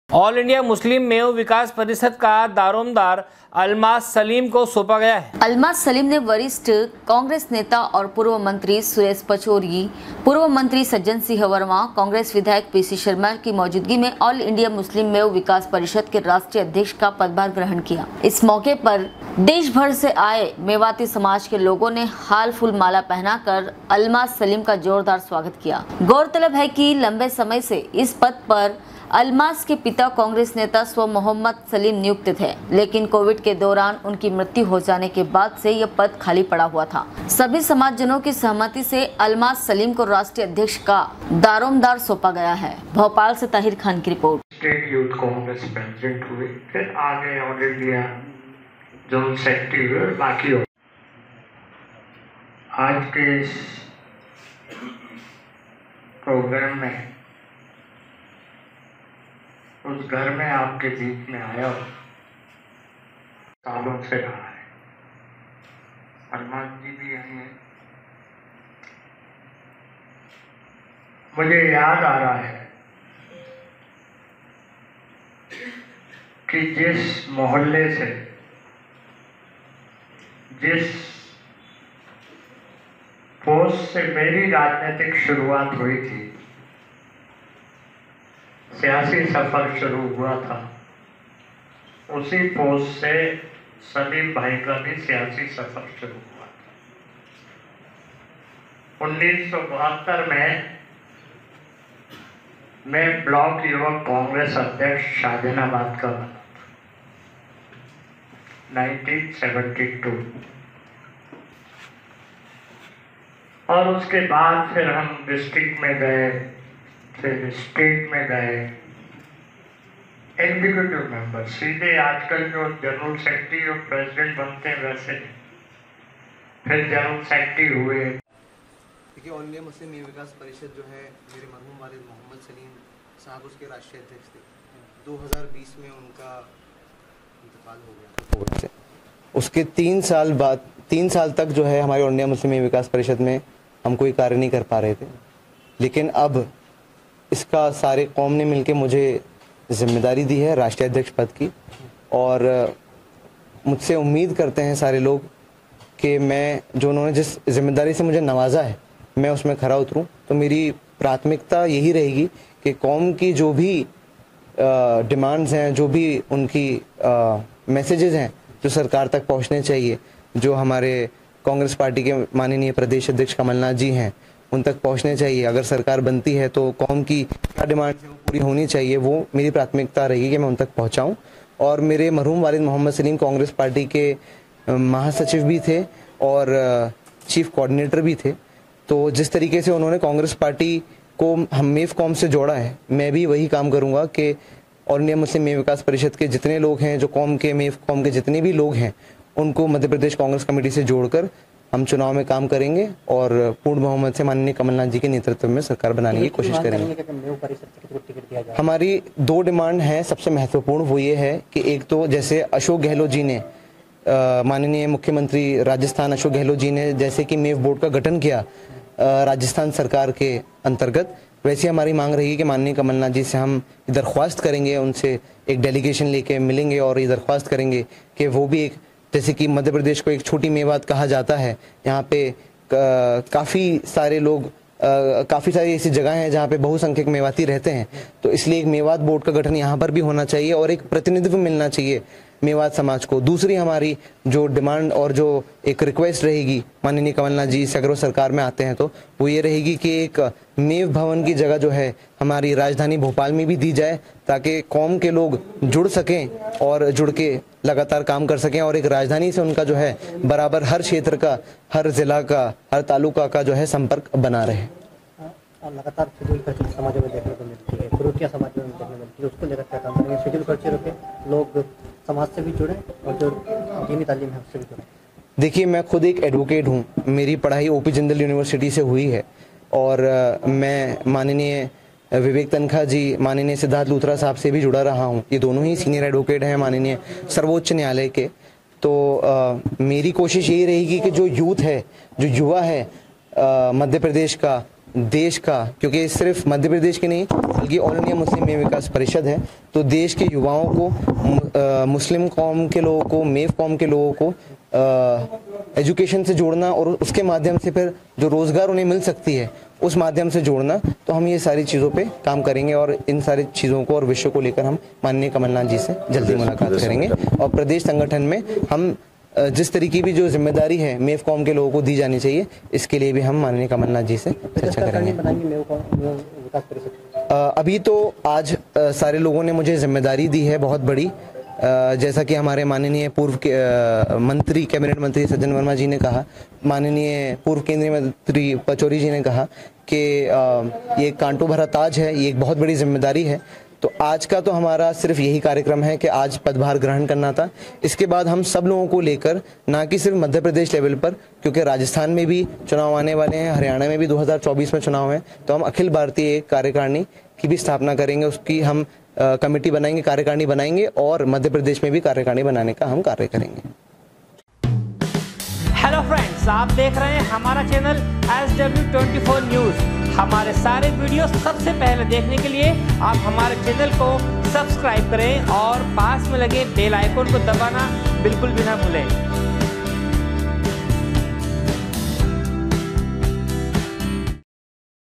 ऑल इंडिया मुस्लिम मेव विकास परिषद का दारोमदार अलमास सलीम को सौंपा गया है.
भोपाल से ताहिर खान की रिपोर्ट।